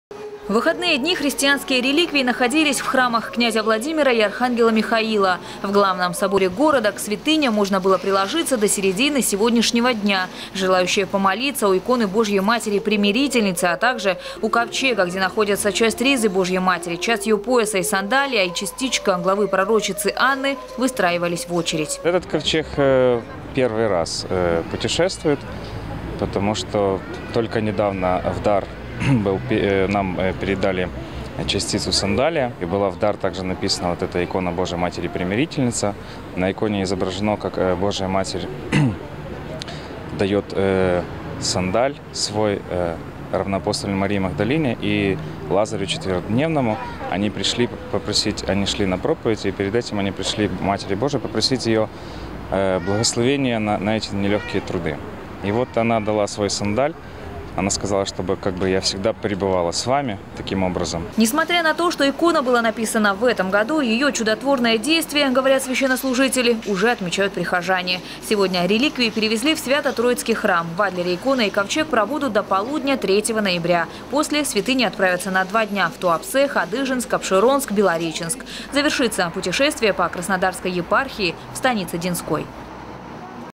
В Сочи находятся святыни из Свято-Ильинского монастыря Одессы. Новости Сочи Эфкате
Реликвии прибыли в кубанскую столицу 21 сентября — в день рождества Богородицы. В Краснодаре икона Богородицы и ковчег с мощами находились две недели. После приложиться к святыням смогли жители станиц, и теперь святыни находятся в Сочи.
Сочи. Новости.
Телеканал Эфкате.